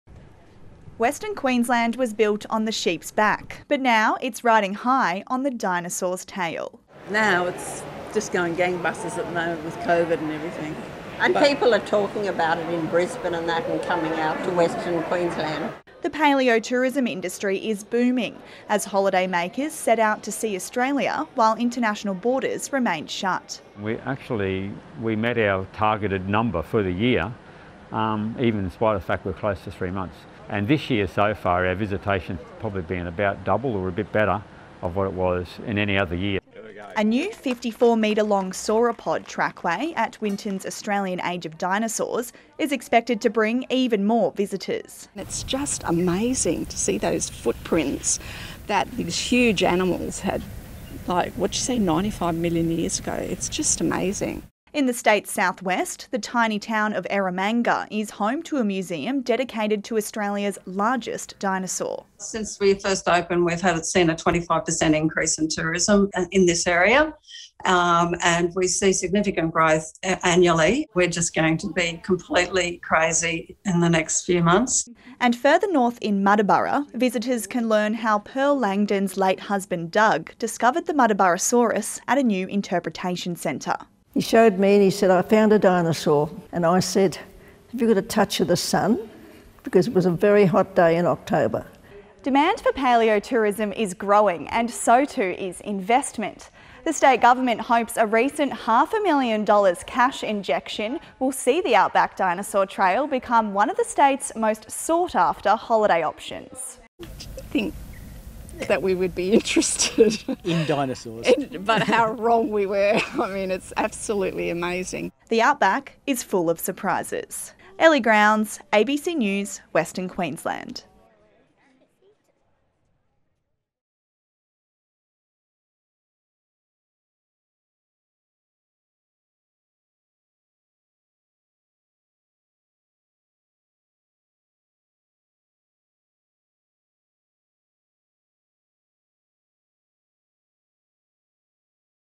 Paleo-tourism on the rise as COVID restrictions sees tourists holidaying in Australia | ABC News
Visitor numbers are booming in outback Queensland to see the footprints and fossils of prehistoric creatures, and the region is positioning itself to become the paeo-tourism capital of the world.